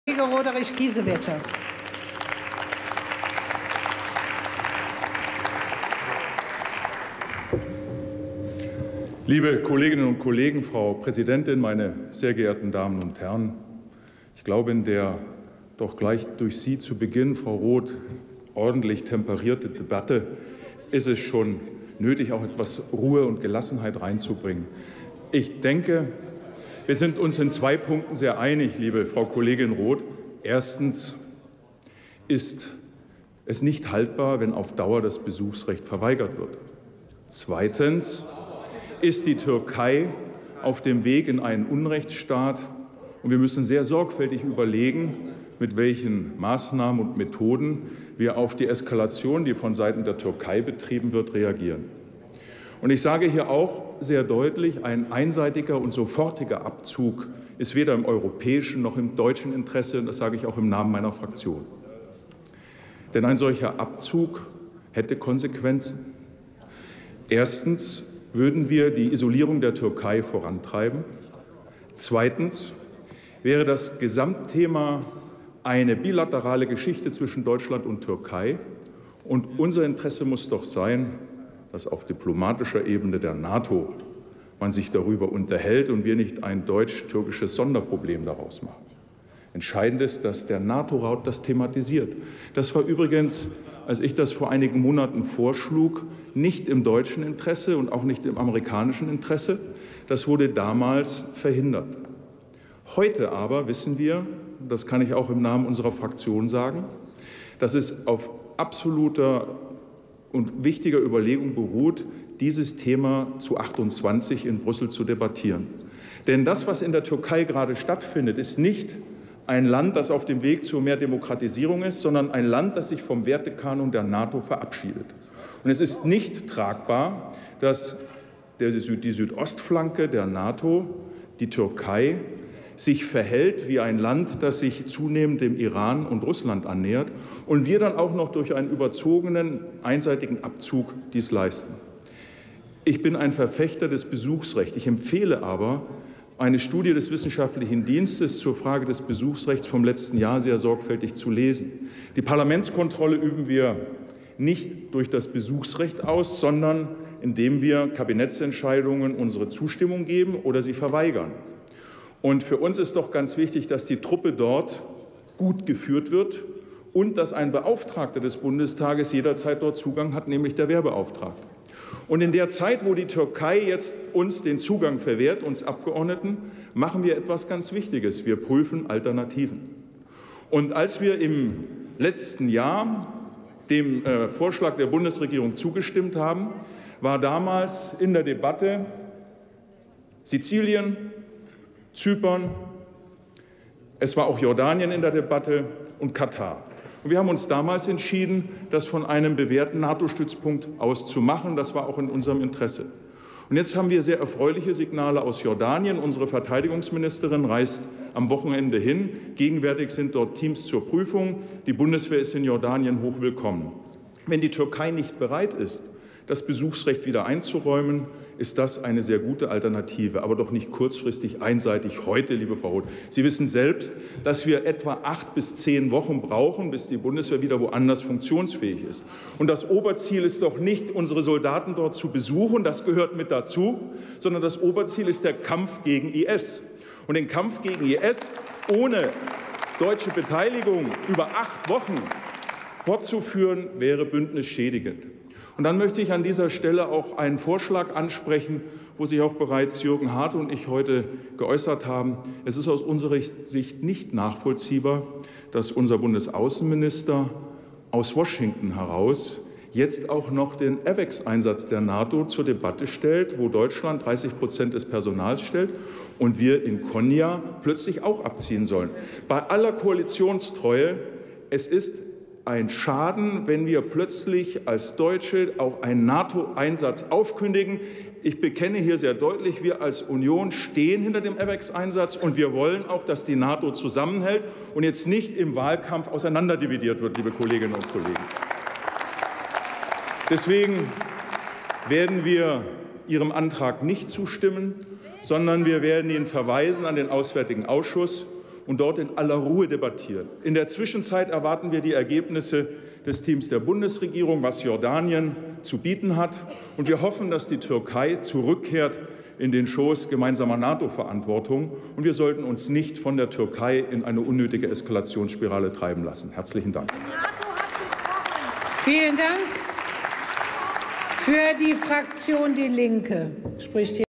Roderich Kiesewetter: Abzug der Bundeswehr aus Incirlik [Bundestag 18.05.2017]
Beratung des Antrags der Fraktionen BÜNDNIS 90/DIE GRÜNEN und DIE LINKE.
Sofortiger Abzug der Bundeswehr aus Incirlik
Drucksache 18/12372

18. Deutscher Bundestag
234. Plenarsitzung
Tagesordnungspunkt: Abzug der Bundeswehr aus Incirlik
Redner: Kiesewetter, Roderich, CDU/CSU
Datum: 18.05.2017
Plenarprotokoll:
UVID:7111140